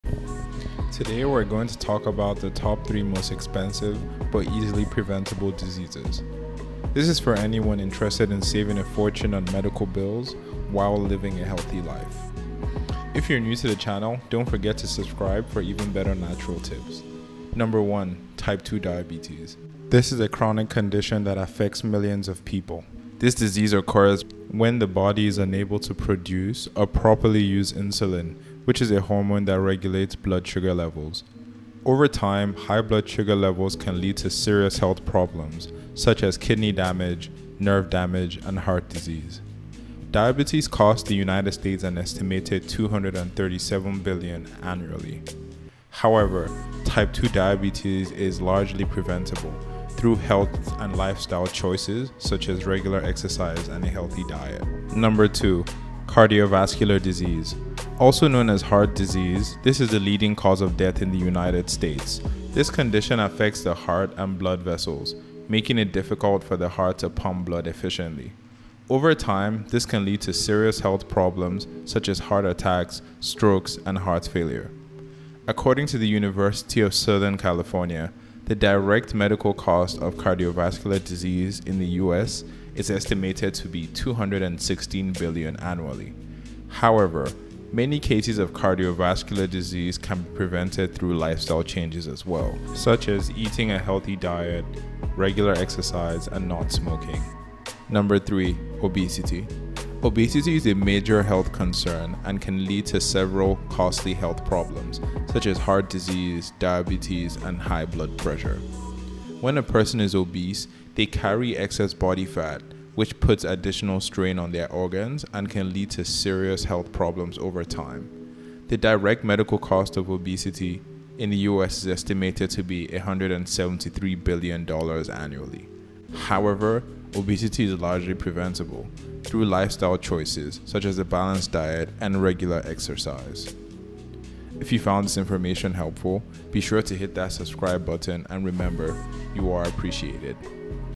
3 most EXPENSIVE illnesses that are FREE to prevent
Today, we're going to talk about the top three most expensive but easily preventable diseases. This is for anyone interested in saving a fortune on medical bills while living a healthy life.